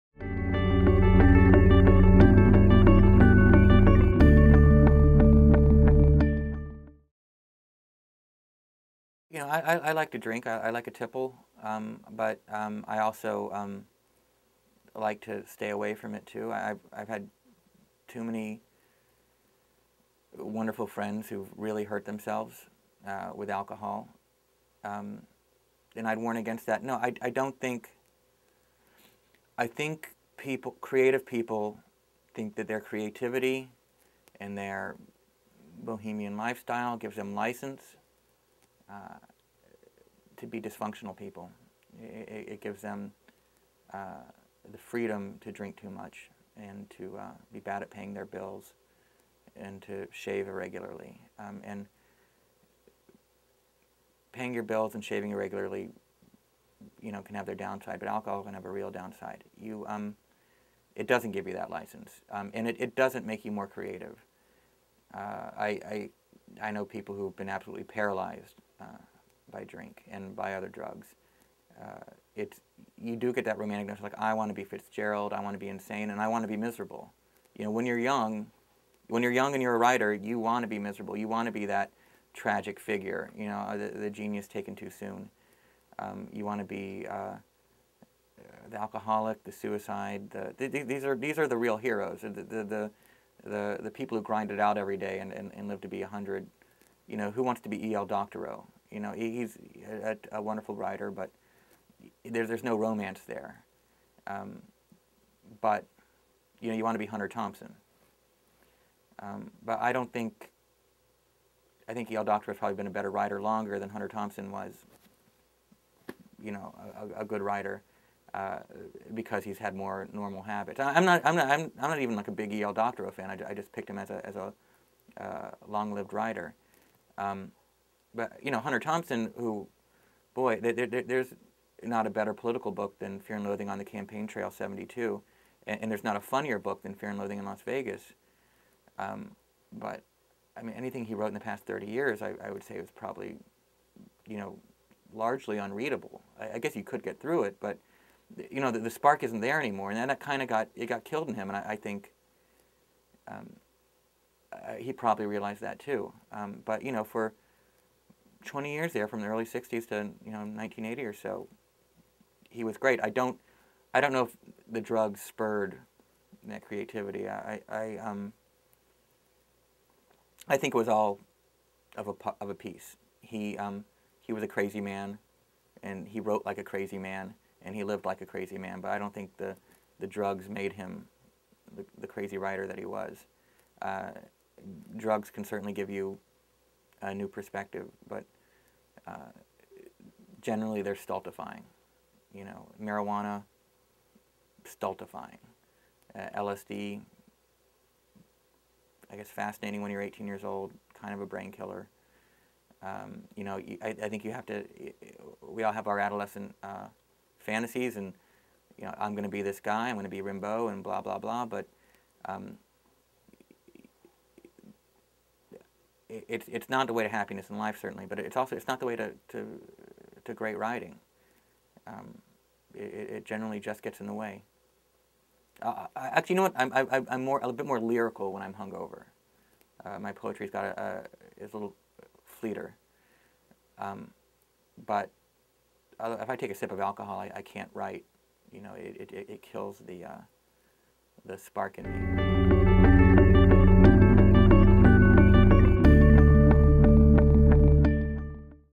Who Wants to be E.L. Doctorow? | Josh Lieb | Big Think
Who Wants to be E.L. Doctorow?
We may all want to be the next drug and booze fueled literary outlaw like Hunter Thompson, but Josh Lieb explains that alcohol leads to early flameouts and difficult lives. E.L. Doctorow, on the other hand, is a model of a long-lived, productive writer.
Josh Lieb:

Josh Lieb is the former Producer and Show Runner of The Tonight Show Starring Jimmy Fallon. His credits include stints as Executive Producer of NewsRadio and The Daily Show with Jon Stewart. He won 7 Prime Time Emmys as a producer and writer for The Daily Show. In 2009, he published a young adult novel, I Am A Genius of Unspeakable Evil and I Want to Be Your Class President, which was a New York Times Bestseller.

Lieb was raised in Columbia, South Carolina, and graduated from Harvard, where he was an editor of The Lampoon, the college humor magazine. After graduation, he found work writing for Twisted Puppet Theater, The Jon Stewart Show, and NewsRadio. He subsequently worked as a producer or consultant on shows including The Simpsons, Drawn Together, Sirens, Nikki, I Hate My Teenage Daughter, and Late Night with Jimmy Fallon.

Lieb's tenure at The Daily Show lasted from 2006 to 2010, during which he also served as Executive Producer of “The Rally to Restore Sanity And/Or Fear” and as co-editor and co-author of Earth: The Book.

In 2013, he wrote and directed a series of comedic shorts to raise money and awareness for the charity Water.Org. Stars featured in the shorts included Matt Damon, Jessica Biel, Sir Richard Branson, and Bono.

Penguin/ Random House released Lieb's second novel, Ratscalibur, in 2015.

In October 2016, NBCUniversal announced an exclusive writing deal with Lieb.
TRANSCRIPT:

Question: Is there a relationship between alcohol and creativity?

Josh Lieb: I like to drink. I like to tipple, but I also like to stay away from it too. I’ve had too many wonderful friends who really hurt themselves with alcohol, and I’d warn against that. No, I don’t think – I think that creative people think that their creativity and their bohemian lifestyle gives them license to be dysfunctional people. It gives them the freedom to drink too much and to be bad about paying their bills and to shave irregularly. And paying your bills and shaving irregularly can have their downside, but alcohol can have a real downside. It doesn’t give you that license, and it doesn’t make you more creative. I know people who have been absolutely paralyzed by drink, and by other drugs. You do get that romantic, like – I want to be Fitzgerald, I want to be insane, and I want to be miserable. When you’re young and you’re a writer, you want to be miserable, you want to be that tragic figure, you know, the genius taken too soon. You want to be the alcoholic, the suicide; these are the real heroes, the people who grind it out everyday and live to be 100. I mean, who wants to be E.L. Doctorow. He was a wonderful writer, but there is no romance there. But, you want to be Hunter Thompson. But I don’t think – I think E.L. Doctorow would have probably been a better writer longer than Hunter Thompson was a good writer because he had more normal habits. I’m not even like a bit E.L. Doctorow fan; I just picked him as a long-lived writer.

But Hunter Thompson who, boy, there’s not a better political book than Fear and Loathing on the Campaign Train ’72, and there’s not a funnier book than Fear and Loathing in Las Vegas, but anything he wrote in the past 30 years I would say is probably largely unreadable. I guess you could get through it, but the spark isn’t there anymore and then it kind of got killed in him and I think he probably realized that too. But for 20 years there, from the early 60’s to 1980 or so, he was great. I don’t know if the drugs spurred that creativity. I think it was all of a piece. He was a crazy man and he wrote like a crazy man, and he lived like a crazy man. But I don’t think the drugs made him the crazy writer that he was. Drugs can certainly give you a new perspective, but generally they’re stultifying. Marijuana, stultifying. LSD, I guess fascinating when you’re 18 years old, kind of a brain killer. I think you have to – we all have our adolescent fantasies and I’m going to be this guy, I’m gonna Rambo and blah, blah, blah. But, it’s not the way to happiness in life certainly, but it’s also not the way to great writing. It generally just gets in the way. You know what? I’m a bit more lyrical when I’m hung over. My poetry is a little fleeter, but if I take a sip of alcohol, I can’t write. It kills the spark in me.